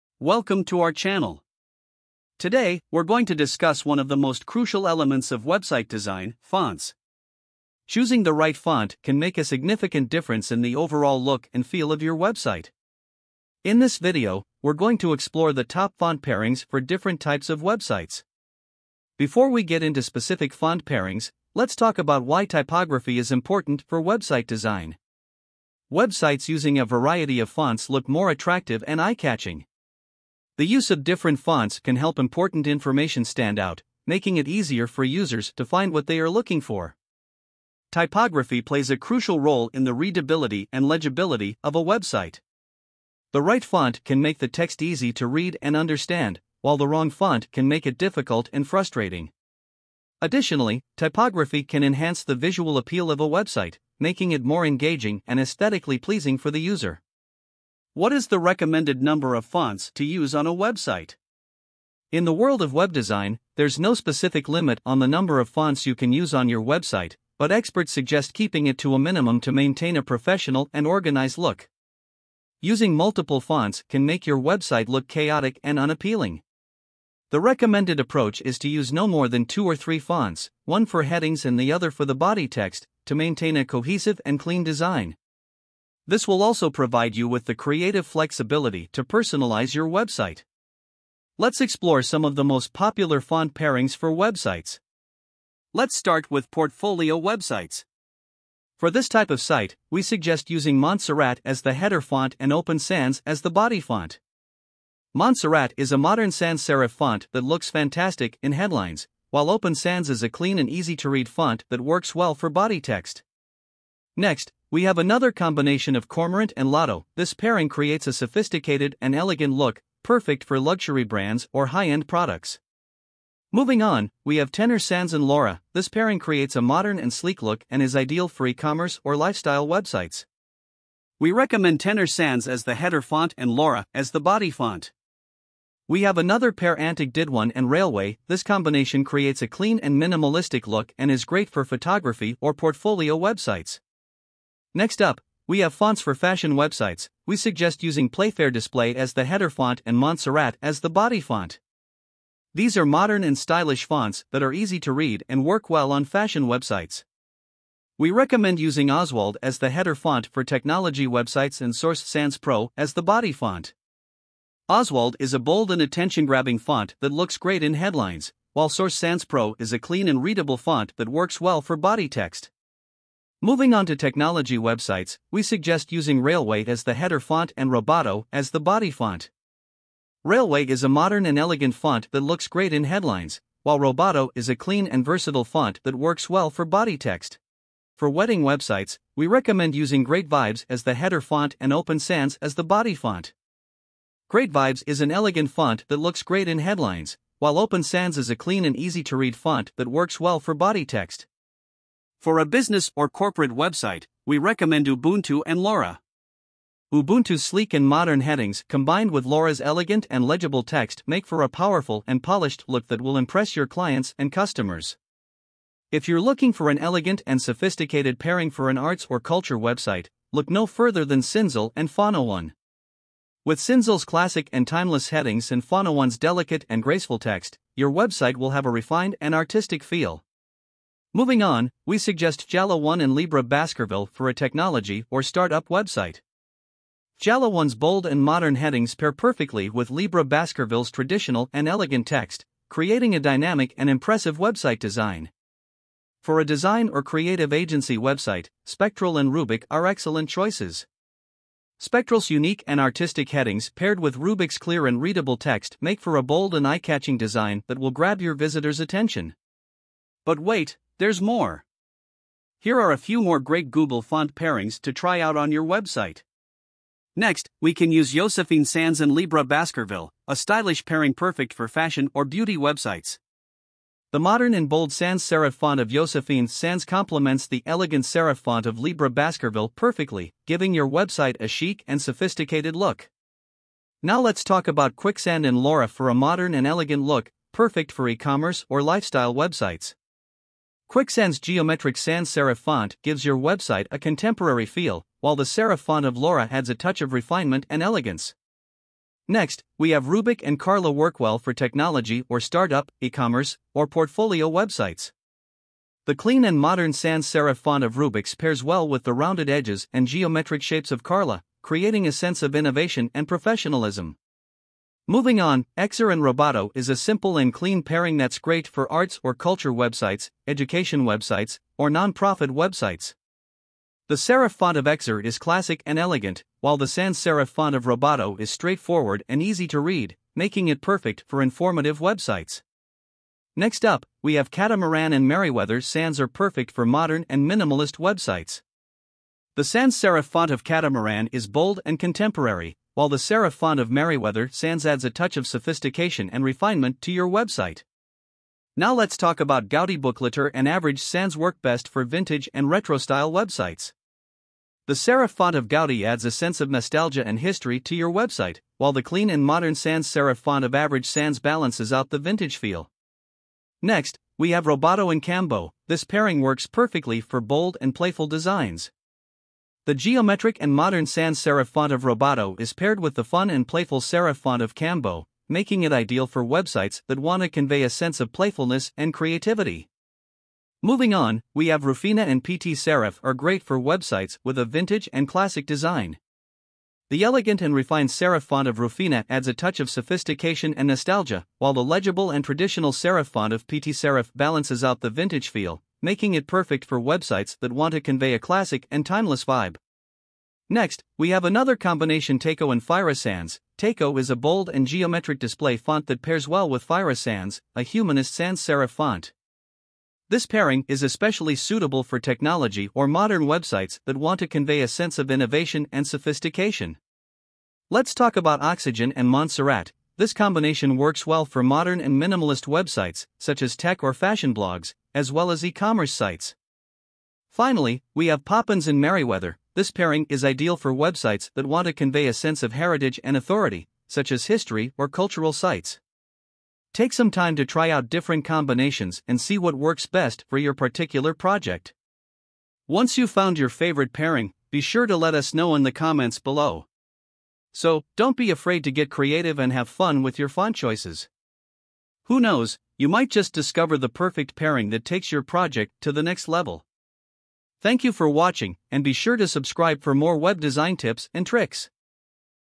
Font Combination for Website - Best Fonts for Designers
This YouTube video is a guide for designers and website developers who want to improve the typography of their website designs. The video discusses font combinations that work well together and offers examples of how to use them effectively.